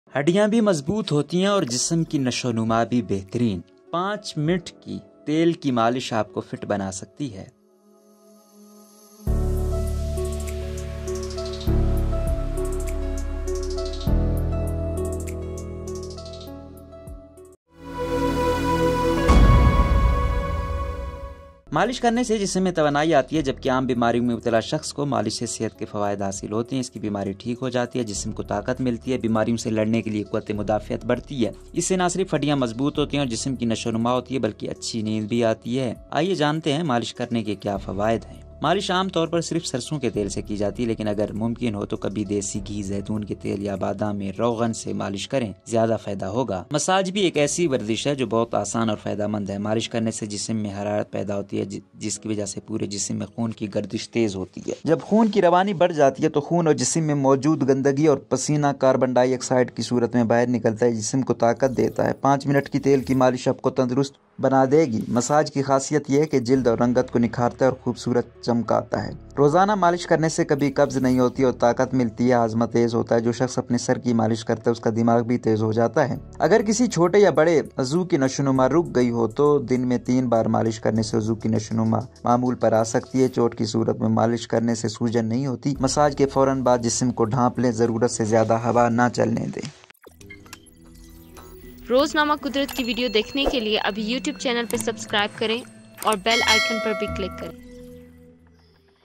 Bones will also strong and body development is also excellent. 5 minute oil massage can make you fit
Bones are also strong and body development is also excellent. 5
 A minute oil massage can make you fit
ہڈیاں بھی مضبوط ہوتی ہیں اور جسم کی نشوونما بھی بہترین ۔۔ 5
 منٹ کی تیل کی مالش آپ کوفِٹ بنا سکتی ہے